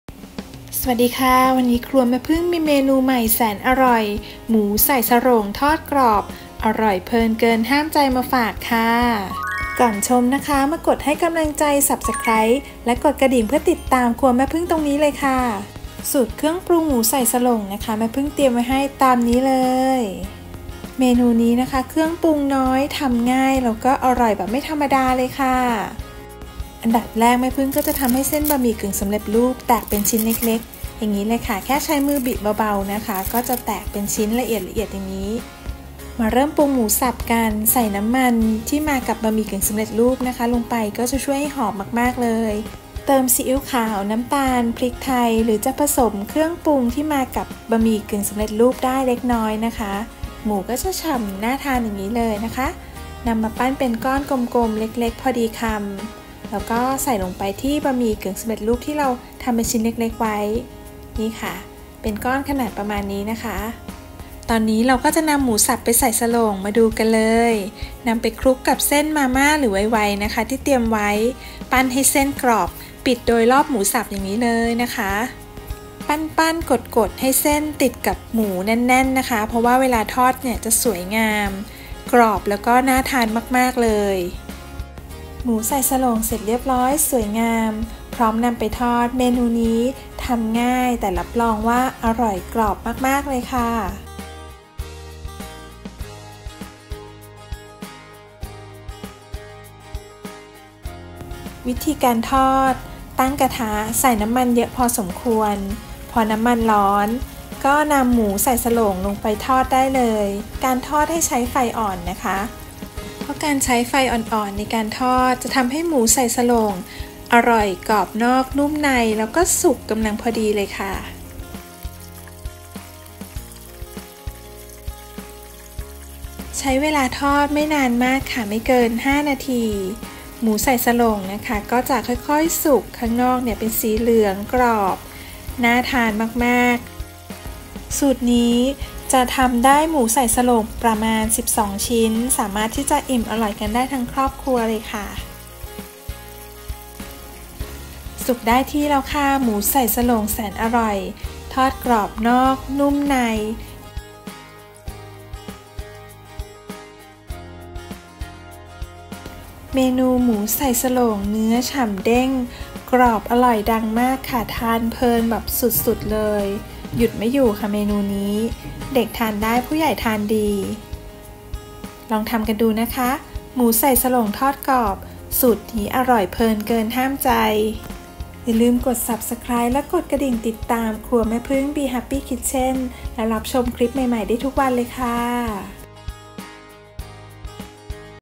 วิธีทำหมูใส่โสร่ง หมูทอดมาม่าไวไวทำง่าย อร่อยกรุบกรอบเพลินเกินห้ามใจ |ครัวแม่ผึ้ง bee happy kitchen
ครัวแม่ผึ้ง bee happy kitchen มีเมนูหมูทอดง่ายๆแบบว่าอร่อยไม่ธรรมาดาจ้ะ
“หมูใส่โสร่งทอดกรอบ“  Fried Crispy pork noodle ball หมูทอดคลุกเส้นมาม่าไวแบบนี้อร่อยสุดหยุดไม่อยู่จริงๆ
บอกเลยว่าเมนูทำแสนจะง่าย แต่อร่อยกรุบกรอบเพลินเกินห้ามใจ

1.หมูสับ 200 กรัม
2.ซีอิ๊วขาว 2 ช้อนชา
3.น้ำตาล 1 ช้อนชา
4.พริกไทย 1/4 ช้อนชา
5.บะหมี่กึ่งสำเร็จรูปมาม่า / ไวไว 1-2 ซอง
6.น้ำมันสำหรับทอด